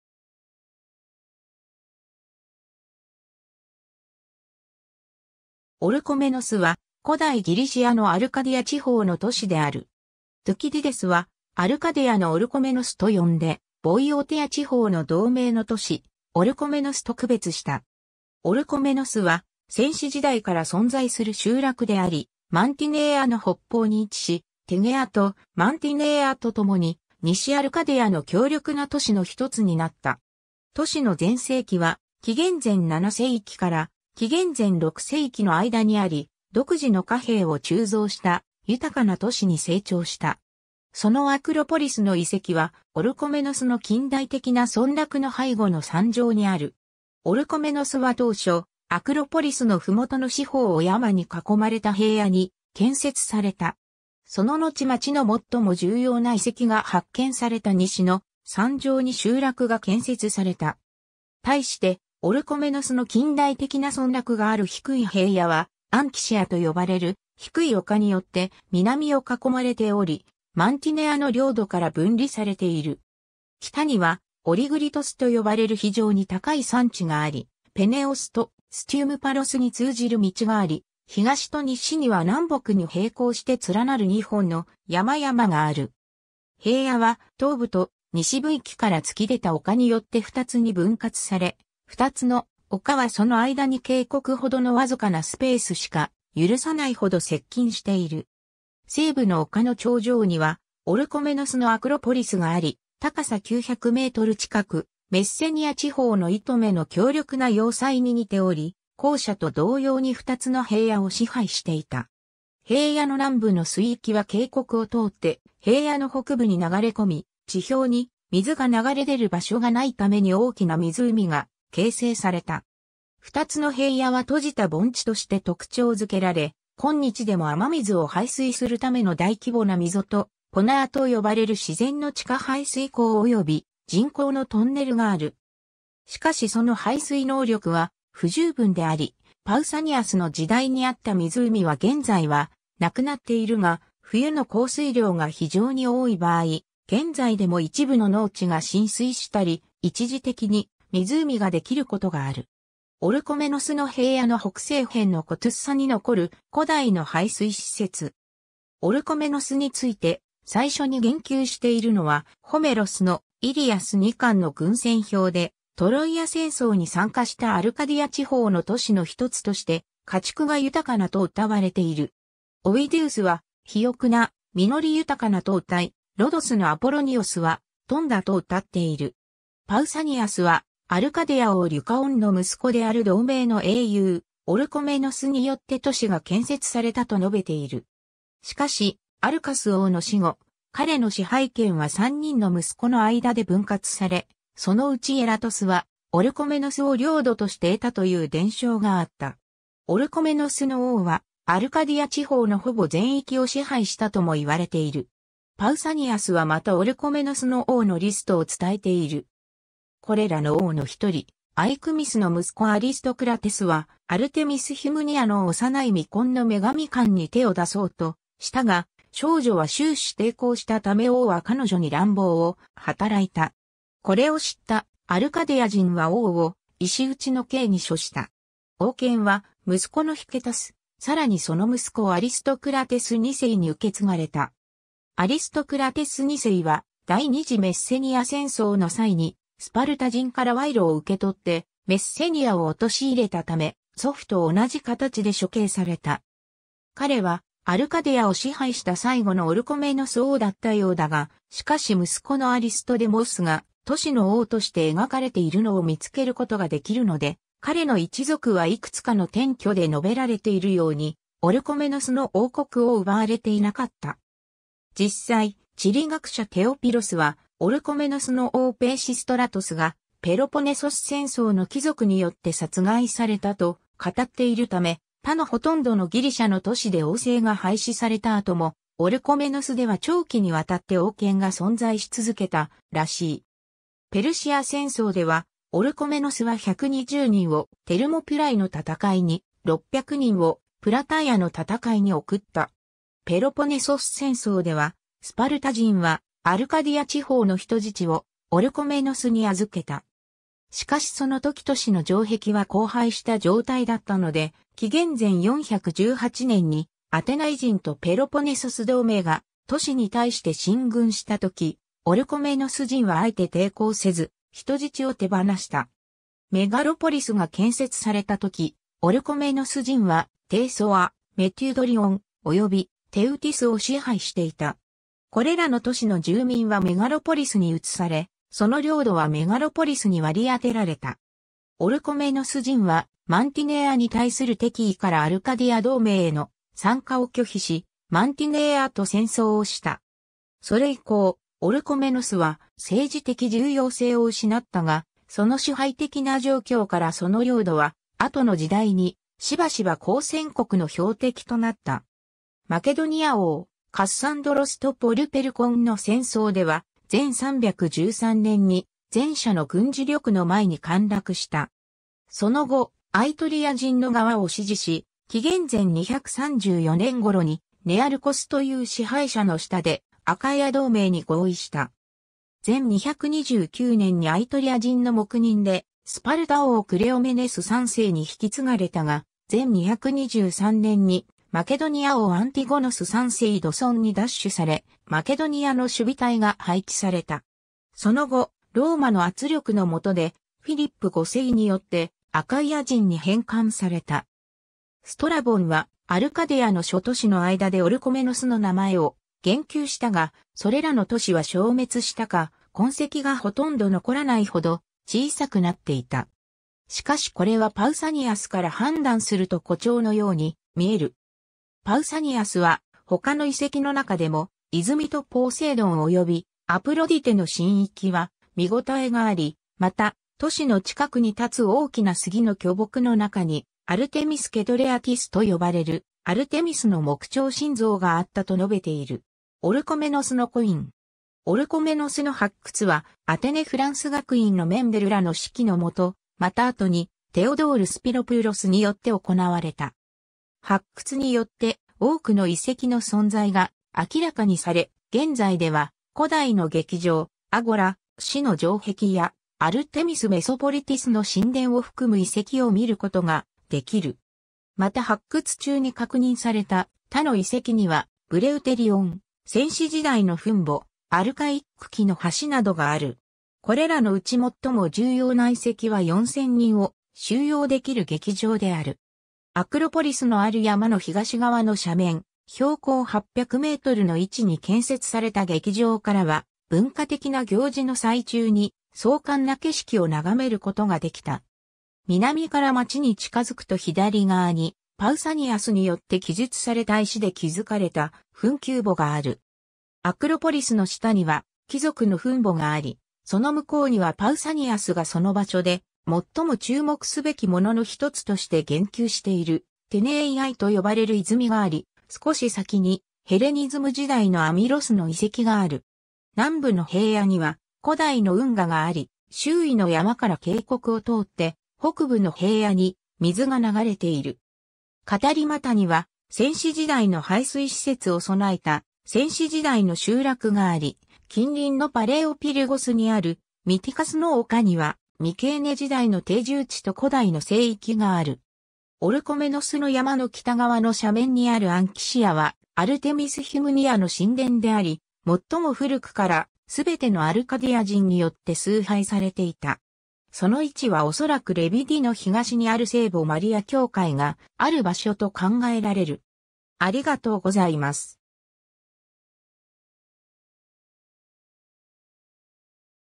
オルコメノス (アルカディア)
オルコメノス（古希: Ὀρχομενός, Orchomenos, 英: Orchomenus）は、古代ギリシアのアルカディア地方の都市である。
トゥキディデスはアルカディアのオルコメノス（古希: Ὀρχομενός ὁ Ἀρκαδικός）と呼んでボイオティア地方の同名の都市オルコメノスと区別した。
 オルコメノスは先史時代から存在する集落であり、マンティネイアの北方に位置し、テゲアとマンティネイアとともに西アルカディアの強力な都市の1つになった。
都市の全盛期は紀元前7世紀から紀元前6世紀の間にあり、独自の貨幣を鋳造した豊かな都市に成長した。
そのアクロポリスの遺跡はオルコメノスの近代的な村落の背後の山上にある。
 オルコメノスは当初、アクロポリスのふもとの四方を山に囲まれた平野に建設された。
そののち町の最も重要な遺跡が発見された西の山上に集落が建設された。
対してオルコメノスの近代的な村落がある低い平野はアンキシア（Anchisia）と呼ばれる低い丘によって南を囲まれており、マンティネアの領土から分離されている。
北にはオリグリトス（Oligyrtus）と呼ばれる非常に高い山地があり、ペネオスとステュムパロスに通じる道があり、東と西には南北に並行して連なる2本の山々がある。
 平野は東部と西部域から突き出た丘によって2つに分割され、2つの丘はその間に渓谷ほどのわずかなスペースしか許さないほど接近している。
西部の丘の頂上にはオルコメノスのアクロポリスがあり、高さ900m（3,000フィート）近く、メッセニア地方のイトメの強力な要塞に似ており、後者と同様に2つの平野を支配していた。
平野の南部の水域は渓谷を通って平野の北部に流れ込み、地表に水が流れ出る場所がないために大きな湖が形成された。
2つの平野は閉じた盆地（カルスト地形の窪地）として特徴づけられ、今日でも雨水を排水するための大規模な溝とポナーと呼ばれる自然の地下排水口および人工のトンネルがある。
しかしその排水能力は不十分であり、パウサニアスの時代（2世紀頃）にあった湖は現在はなくなっているが、冬の降水量が非常に多い場合、現在でも一部の農地が浸水したり、一時的に湖ができることがある。
 オルコメノスの平野の北西辺のコトゥッサ（Chotoussa）に残る古代の排水施設。
 オルコメノスについて最初に言及しているのはホメロスの『イリアス』2巻の軍船表で、トロイア戦争に参加したアルカディア地方の都市の1つとして、「家畜が豊かな（ポリュメロス、πολύμηλος, polymelos）」と歌われている。
オウィディウスは「肥沃な、実り豊かな（ferax）」と歌い、ロドスのアポロニオスは「富んだ」と歌っている。
 パウサニアスはアルカディア王リュカオンの息子である同名の英雄オルコメノスによって都市が建設されたと述べている。
しかしアルカス王の死後、彼の支配権は3人の息子の間で分割され、そのうちエラトスはオルコメノスを領土として得たという伝承があった。
オルコメノスの王はアルカディア地方のほぼ全域を支配したとも言われている。
パウサニアスはまたオルコメノスの王のリストを伝えている。
これらの王の1人、アイクミスの息子アリストクラテスはアルテミス・ヒュムニア（賛歌祭のアルテミス）の幼い未婚の女神官に手を出そうとしたが、少女は終始抵抗したため王は彼女に乱暴を働いた。
これを知ったアルカディア人は王を石打ちの刑に処した。
王権は息子のヒケタス、さらにその息子アリストクラテス2世に受け継がれた。
アリストクラテス2世は第二次メッセニア戦争の際にスパルタ人から賄賂を受け取って、メッセニアを陥れたため、祖父と同じ形で処刑された。
彼はアルカディアを支配した最後のオルコメノス王だったようだが、しかし息子のアリストデモスが都市の王として描かれているのを見つけることができるので、彼の一族はいくつかの典拠で述べられているようにオルコメノスの王国を奪われていなかった。
実際、地理学者テオピロスは、オルコメノスの王ペイシストラトスがペロポネソス戦争の貴族によって殺害されたと語っているため、他のほとんどのギリシャの都市で王政が廃止された後もオルコメノスでは長期にわたって王権が存在し続けたらしい。
 ペルシア戦争では、オルコメノスは120人をテルモピュライの戦いに、600人をプラタイアの戦いに送った。
ペロポネソス戦争では、スパルタ人はアルカディア地方の人...